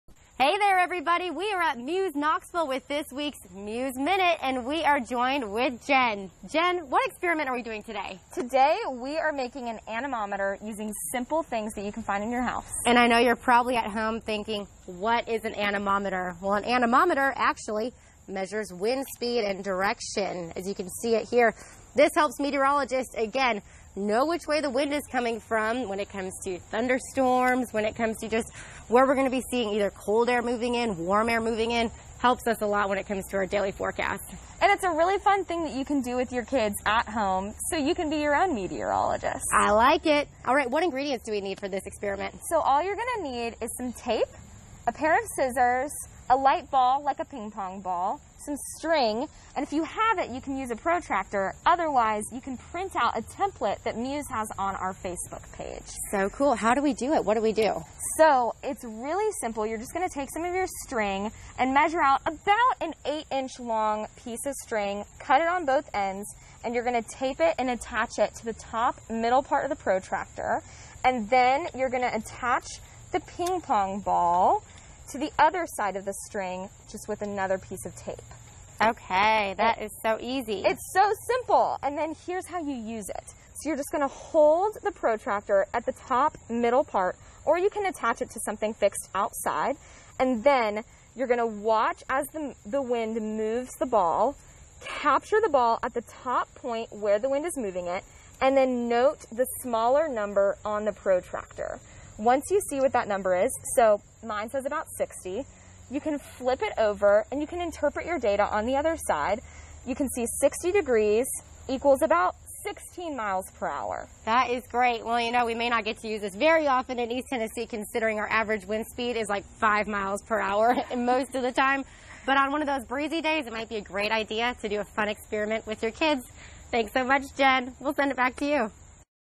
Making An Anemometer - Easy Weather for Kids
Grab scissors, yarn, a ping pong ball and a protractor.